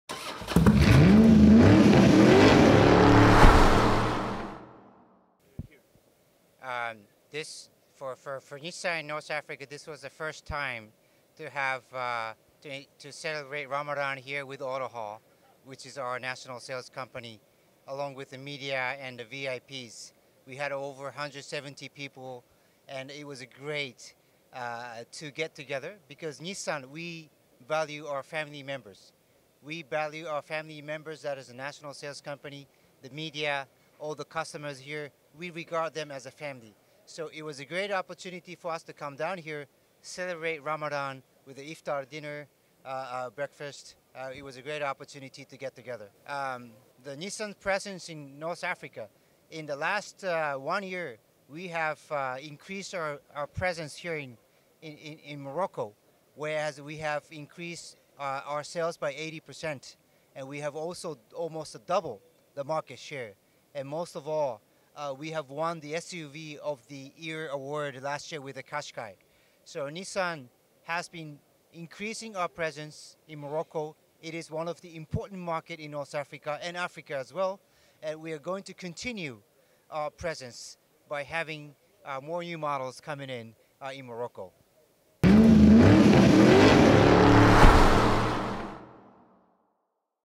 Nissan : Entretien avec Isao Sekiguchi, président de Nissan Afrique du Nord et Egypte | Mekanikus
Durant le GRANFTOURISMO organisé par Nissan à Casablanca jeudi dernier, Isao Sekiguchi, président de Nissan Afrique du Nord et Egypte, nous confie ses impressions sur la présence du groupe au Maroc. Il nous annonce également que la Nissan Murano version 2016 arrivera bientôt au Maroc !